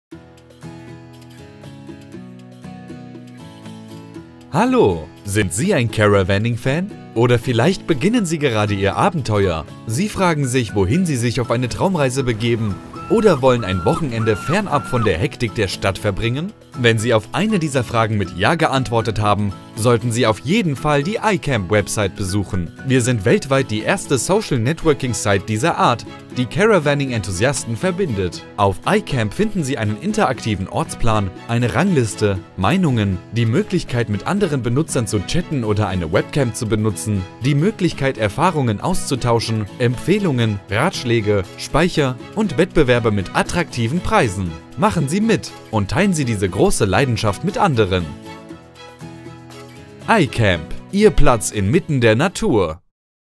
iCamp.de - Portal für Caravaning-Fans
Die Eröffnung des größten sozialen Netzwerks für Caravaning-Fans in Europa steht kurz bevor! Neben einer interaktiven Karte der Orte finden Sie nützliche Artikel zum Thema Caravaning, Straßenverkehrsordnung, einen Abschnitt über den Verkauf und die Vermietung von Wohnmobilen und Anhängern, das iCamp-Forum - ein Ort, um Erfahrungen und Ideen auszutauschen oder sich mit Gleichgesinnten zu unterhalten, Wettbewerbe mit attraktiven Preisen und vieles mehr!